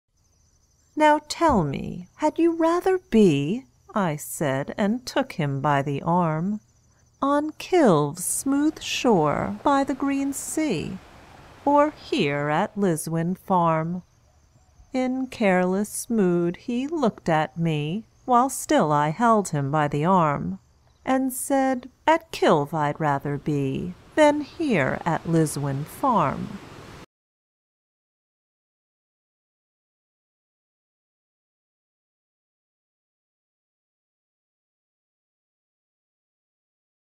William Wordsworth - Anecdote for Fathers (Animation)
Poem: William Wordsworth
Voice: Rhonda Federman (Librivox)
Direction: Farnood

Links to our Amazon books:
The Story of Ulysses Cat
Meet the Olympians